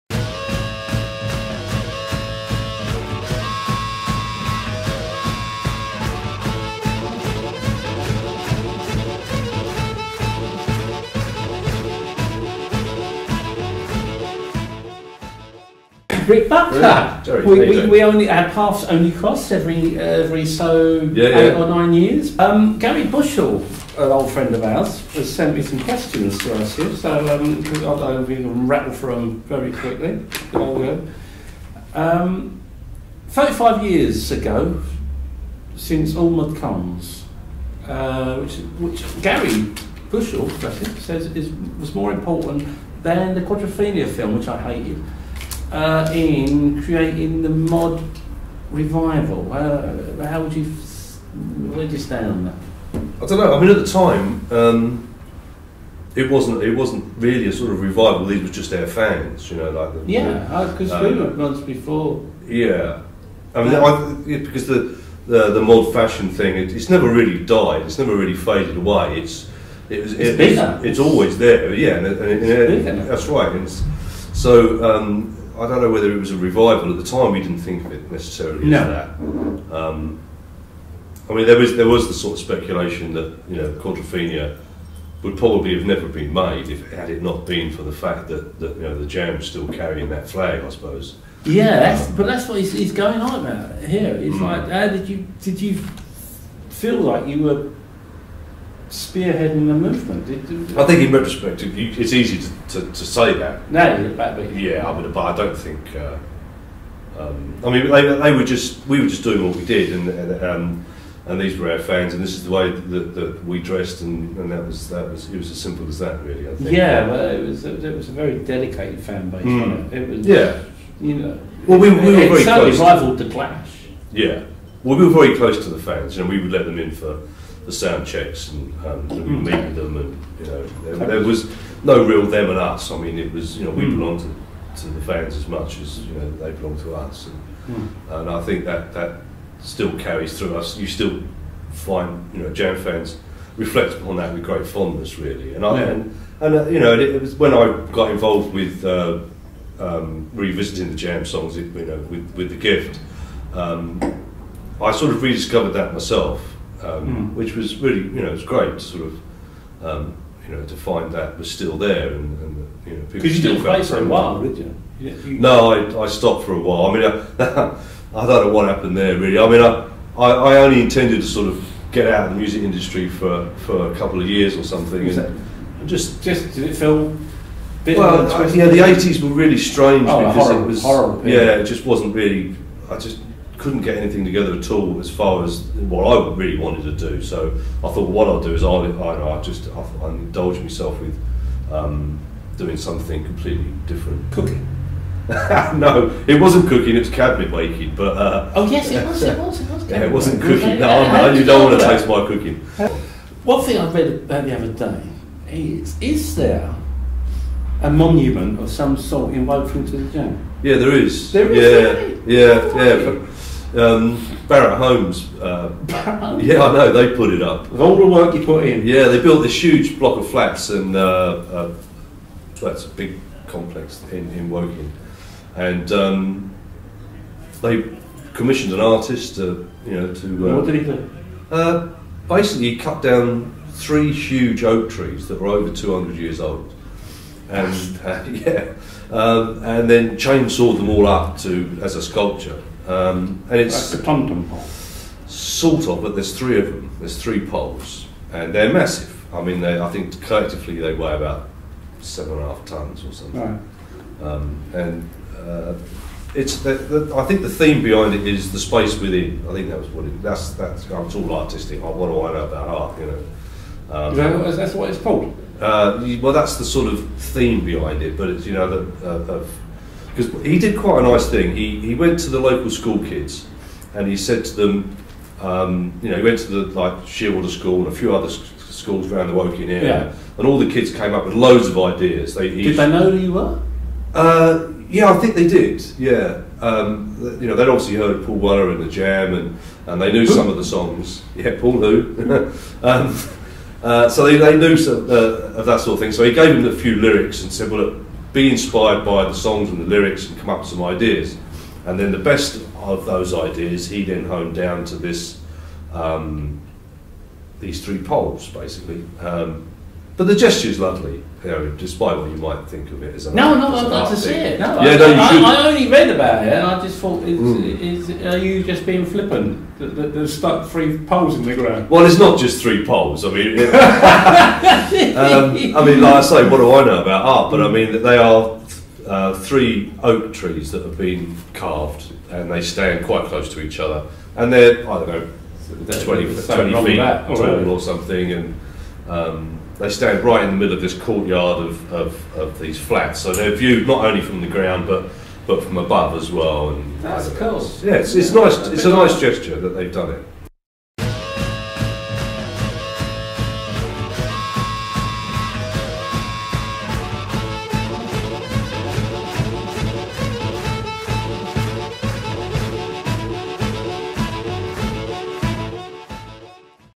Terry talks to Rick Buckler - Part One
Rick Buckler talks to Terry about his time with The Jam and explains about the sculpture of the band in Woking